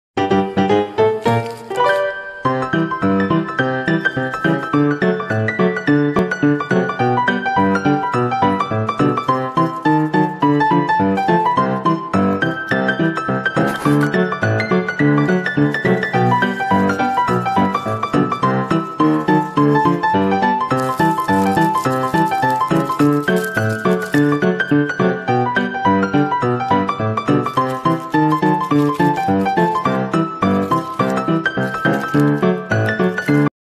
REUSE PARROT FOIL BALLOONS
use big straw and slowly press it easy and simple 🥰🥰🥰